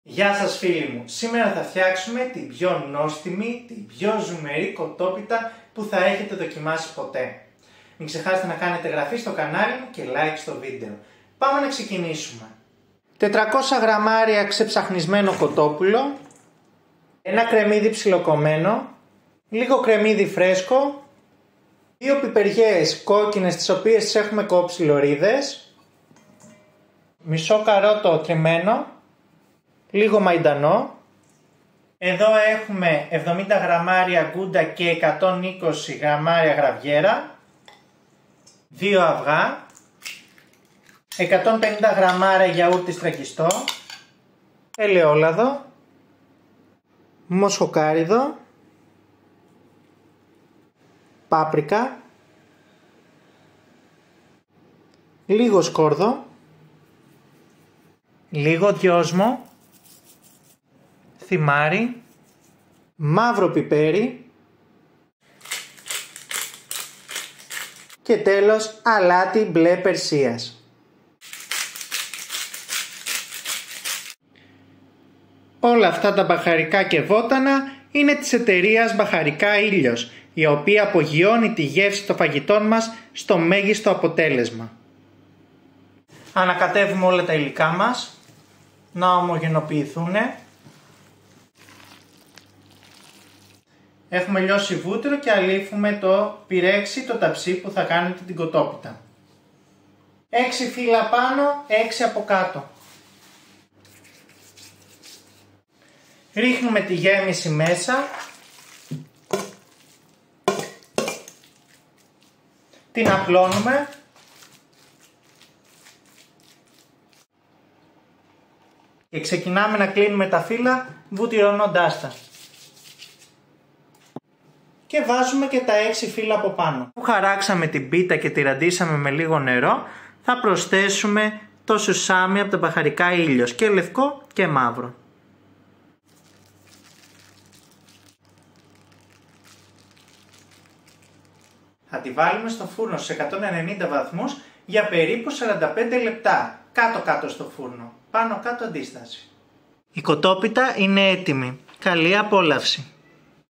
Κοτόπιτα Ζουμερή και Πεντανόστιμη
Σήμερα φτιάχνουμε απίστευτη κοτόπιτα Ζουμερή και Πεντανόστιμη!!! Εύκολη για να την φτιάξει ο καθένας!!!

Υλικά

400 γρ κοτόπουλο ξεψαχνισμενο
1 ξερό κρεμμύδι ψιλοκομμένο
1 φρέσκο κρεμμυδάκι ψιλοκομμένο
2 κόκκινες πιπεριές κομμένες σε λεπτές λωρίδες
Μια χούφτα ψιλοκομμένο μαϊντανό
Μισό καρότο τριμμένο
70 γρ τυρί γκούντα τριμμένο
120 γρ γραβιέρα τριμμένη
2 αυγά
150 γρ γιαούρτι στραγγιστό
Ελαιόλαδο
Μοσχοκάρυδο
Πάπρικα
Λίγο σκόρδο σκόνη
Λίγο δυόσμο
Θυμάρι
Πιπέρι μαύρο τριμμένο
Αλάτι μπλε Περσίας

Μεγάλος Χορηγός Μπαχαρικά Ήλιος

Presentation: Kwnstantinos Tsixlas